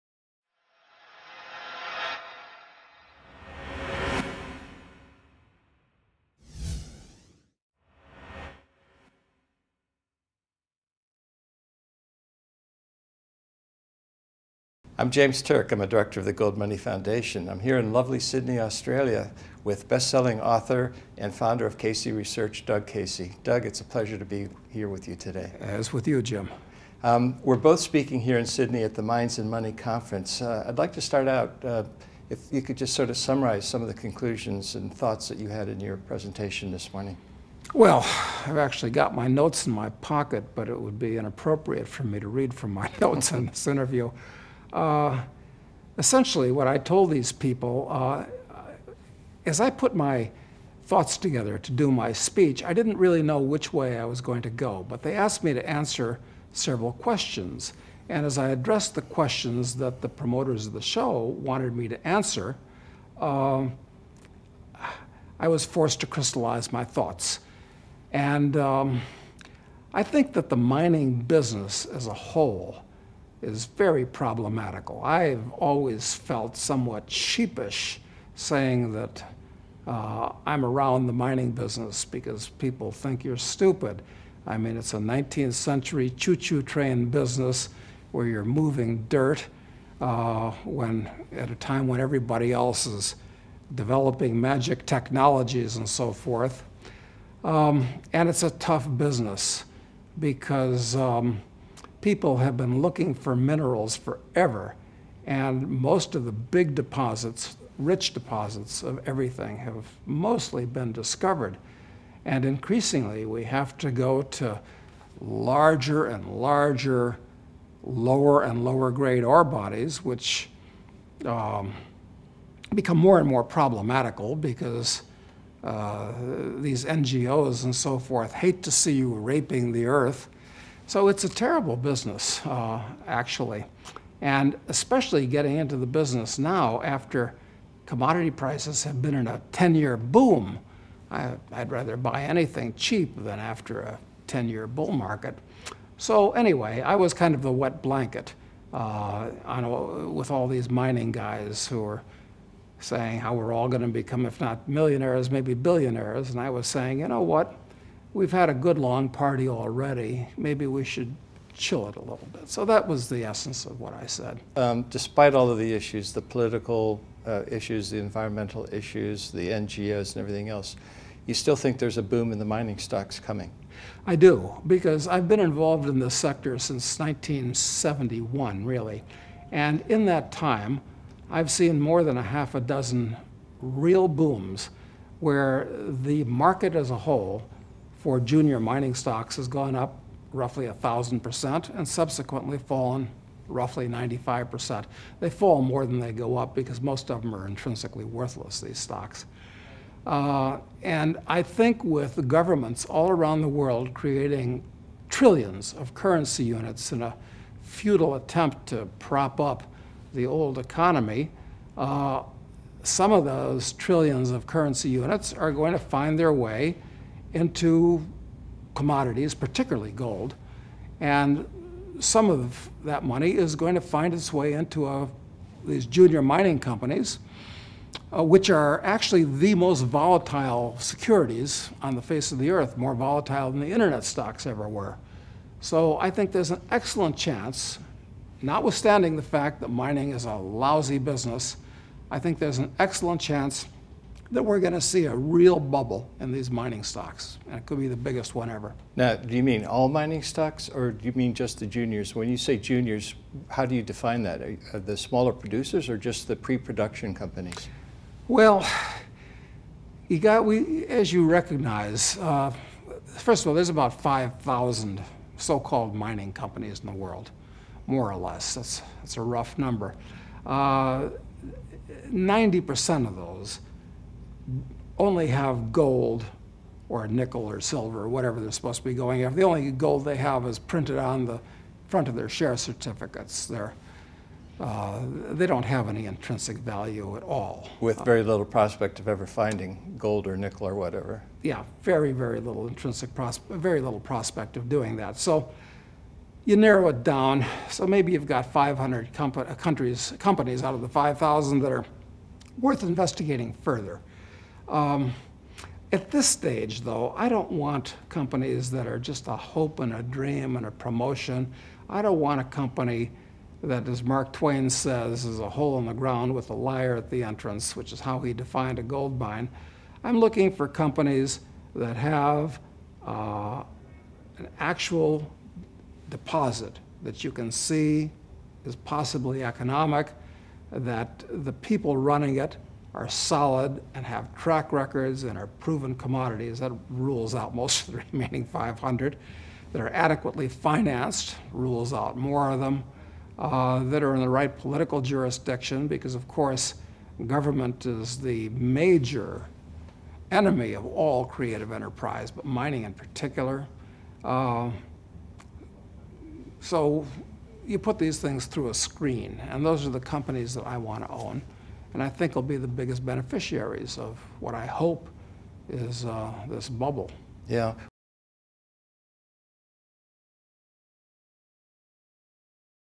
The coming bull market in mining shares
In this video, Doug Casey, founder and chairman of Casey Research Institute, talks to the GoldMoney Foundation's James Turk about the opportunities to be found in mining shares. Though generally bullish on the mining sector, Doug says that finding the right companies to invest in is difficult. He favours the small-to-medium sized producers over the "majors". This interview was recorded in Sydney, Australia in October 2011.